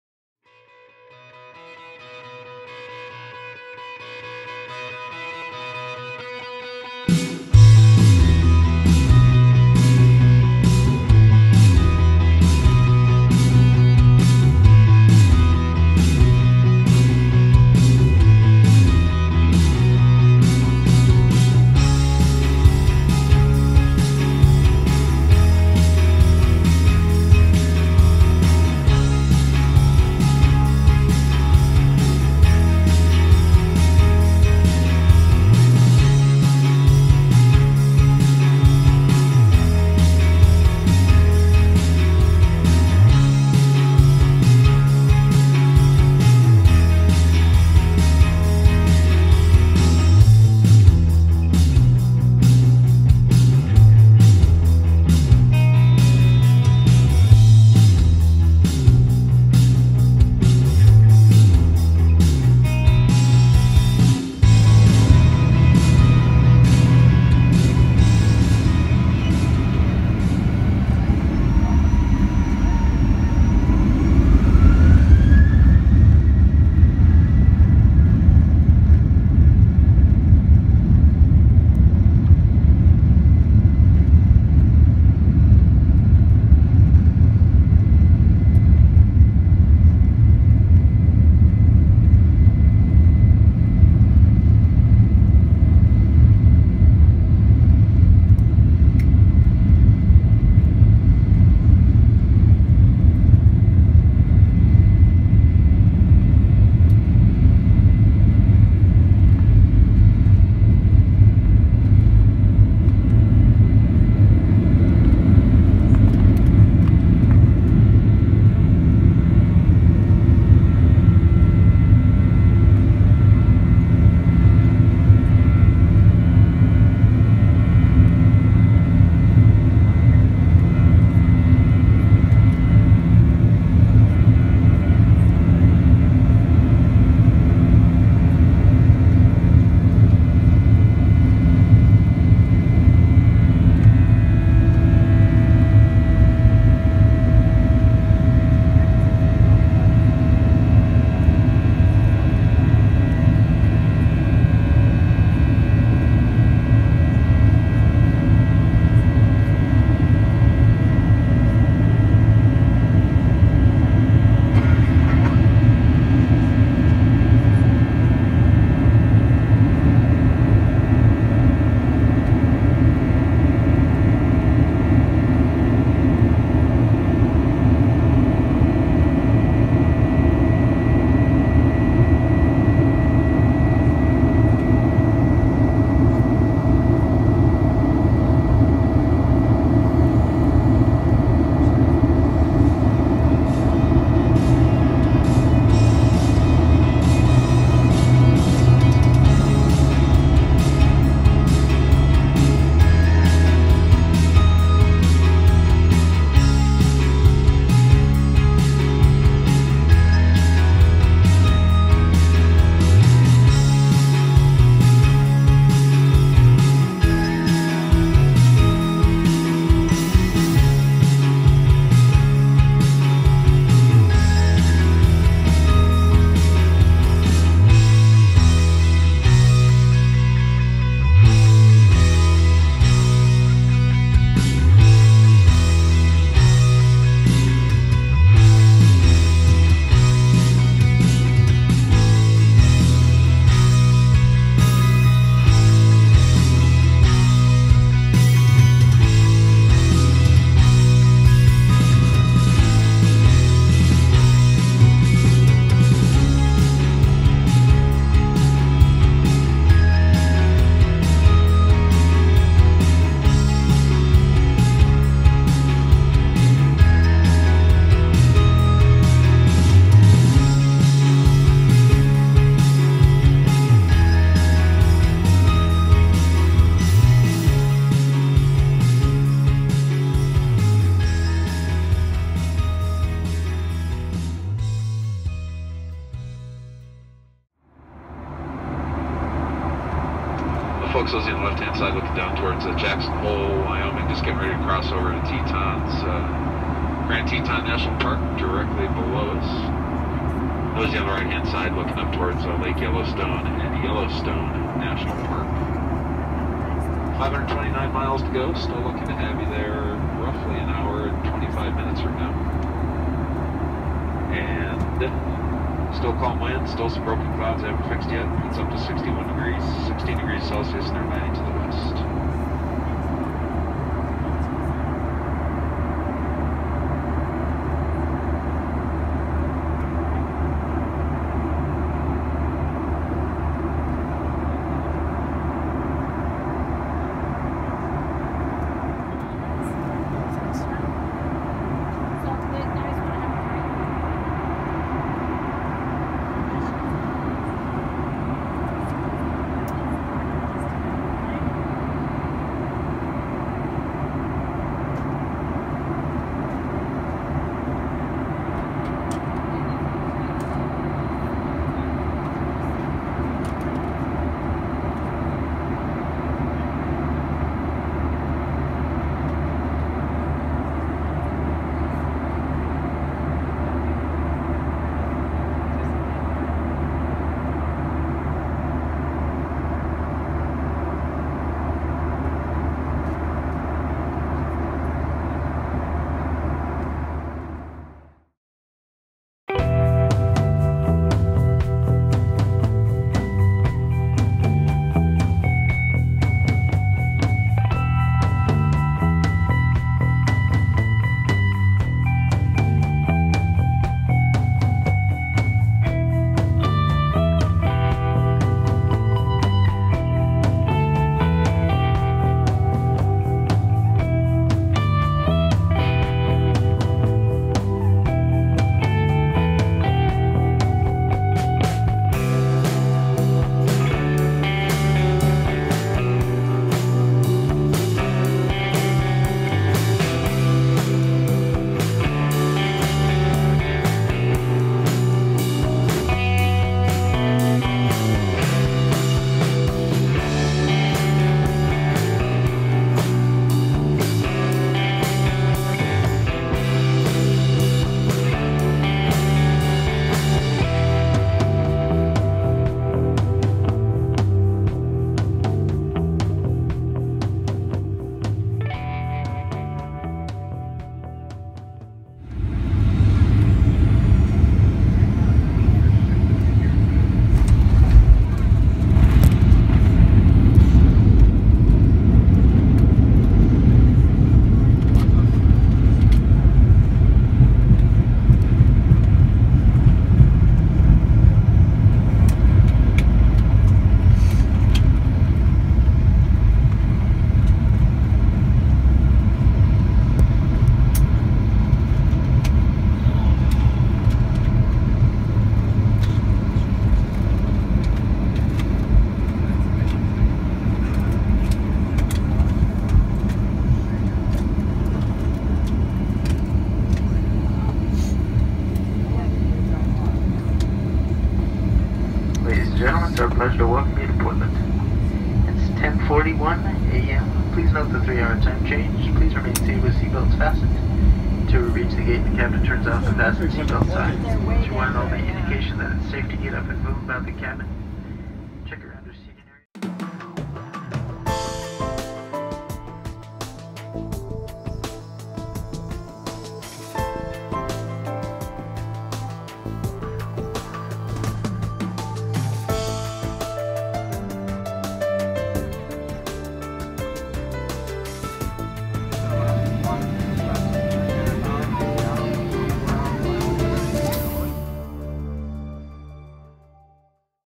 Trip on Alaska Airlines Boeing 737-900ER First Class ATL-PDX
In this video, you will see me flying Alaska Airlines in a first class seat on Boeing 737-900ER, the first class breakfast service, flying by Yellowstone / Grand Teton National Park, and landing in PDX along the Columbia River. Enjoy!